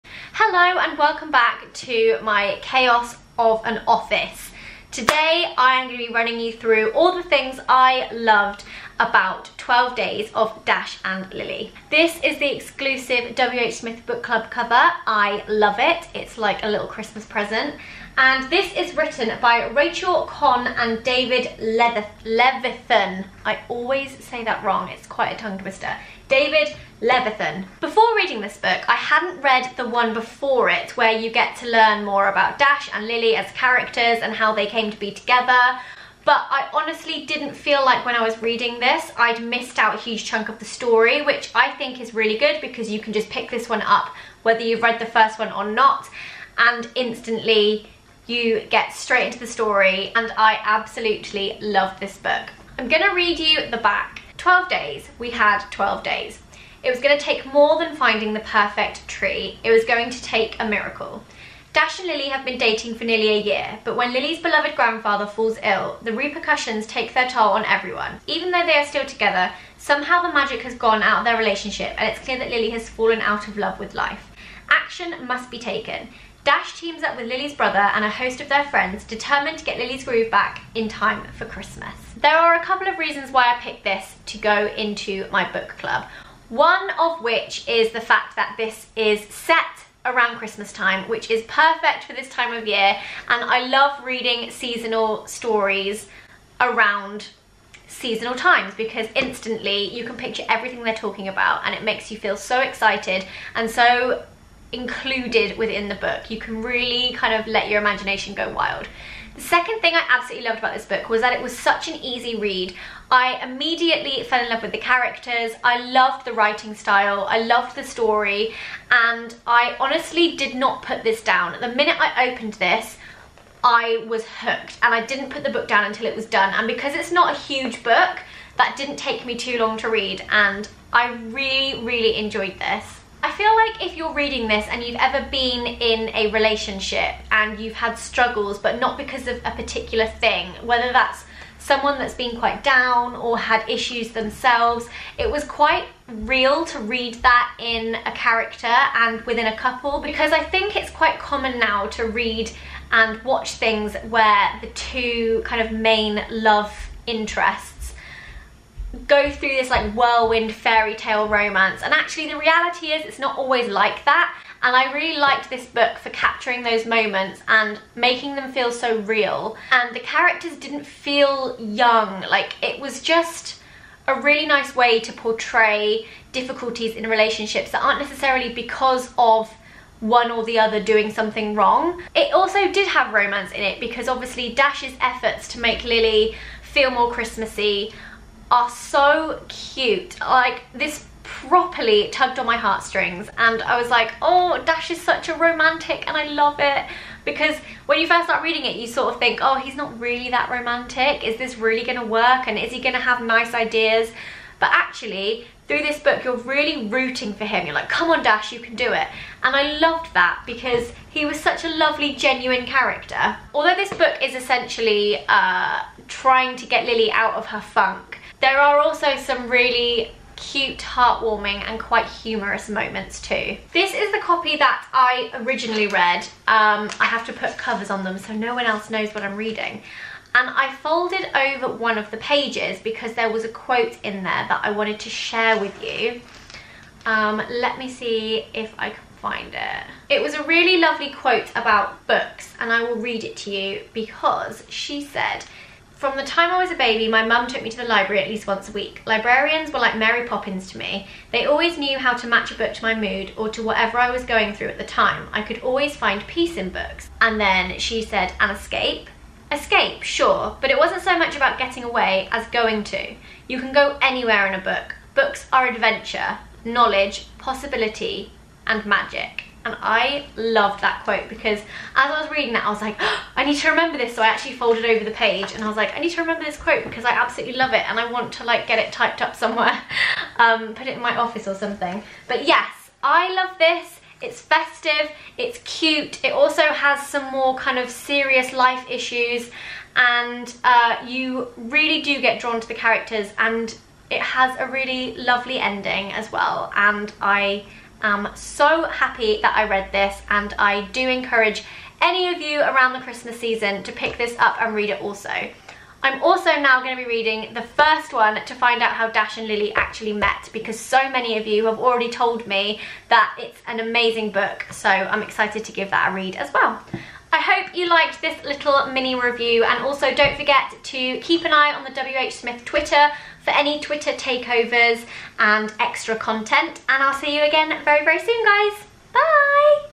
Exclusive! Zoella Reviews The Twelve Days of Dash and Lily by Rachel Cohn and David Levithan
In this exclusive review for the Zoella Book Club Exclusive to WHSmith, Zoe introduces us to The Twelve Days of Dash and Lily by Rachel Cohn and David Levithan. Explaining why she chose it for the book club, Zoe also tells us why she loves seasonal books and shares her favourite quote from the book.

A teen romance set against the magical backdrop of New York City in December. It is the ultimate Christmas book. Dash and Lily have been dating for nearly a year, but when Lily's beloved grandfather falls ill, the repercussions take their toll on everyone. Even though they are still together, somehow the magic has gone out of their relationship and it's clear that Lily has fallen out of love with life. Action must be taken! Dash teams up with Lily's brother and a host of their friends, who have just twelve days to get Lily's groove back in time for Christmas. This is a warm, wintry read that is guaranteed to be a favourite Christmas book for many years to come.